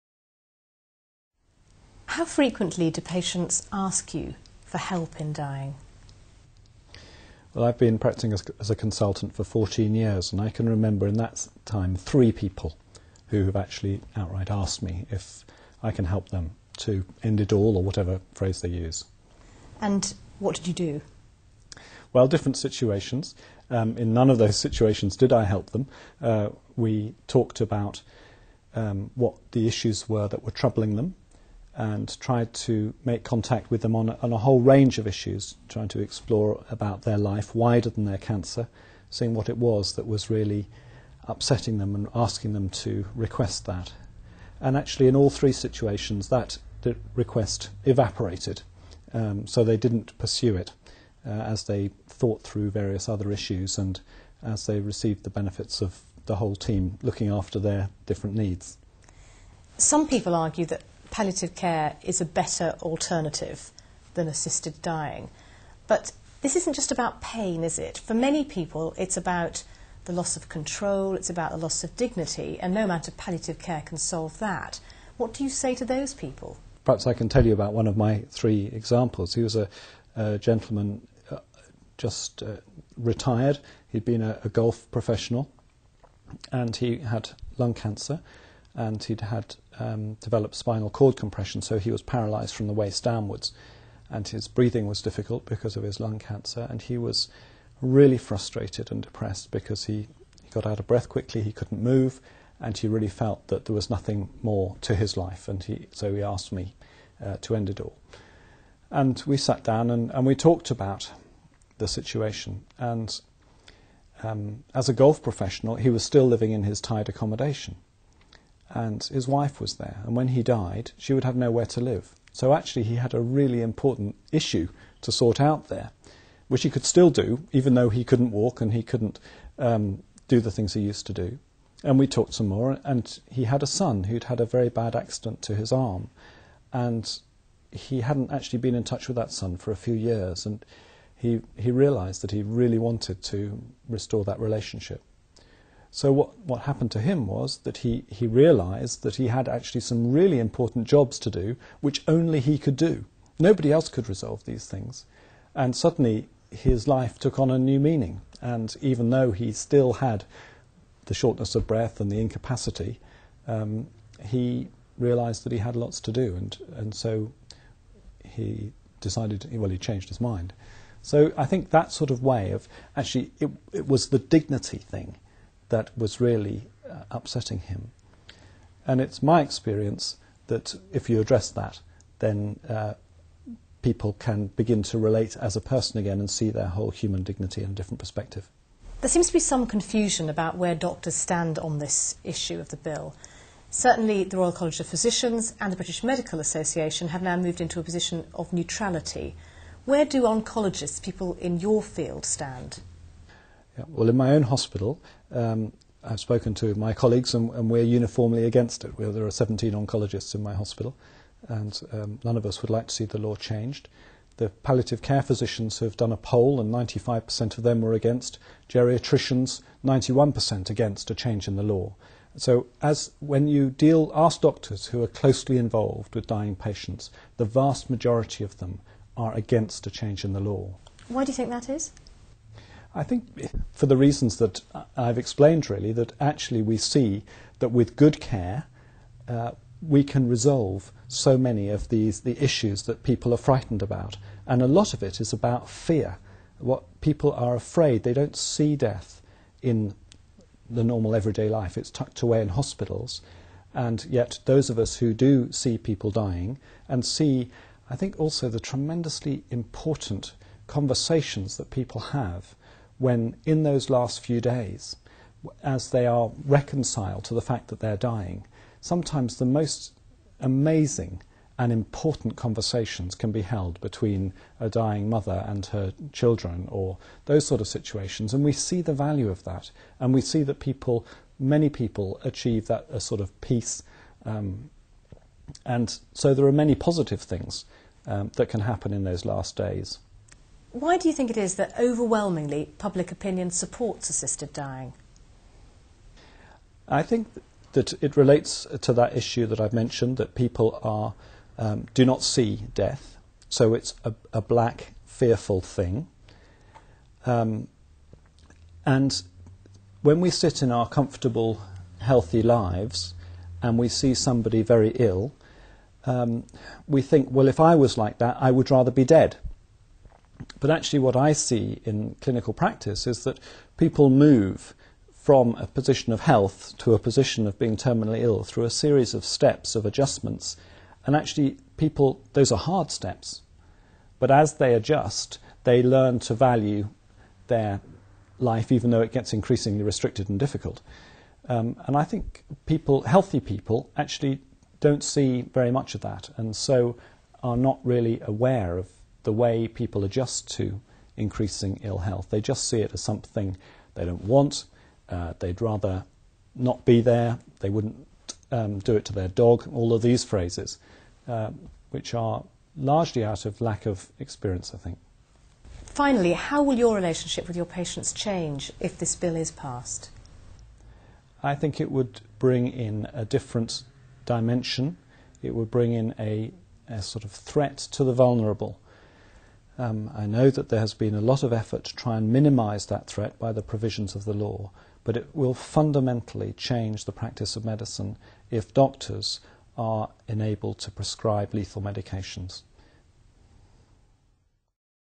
Care Not Killing - euthanasia interviews - an oncologist speaks
Prof Tim Maughan, Consultant Clinical Oncologist:
"In my own hospital we are uniformly against [assisted dying]. None of us would like to see the law changed."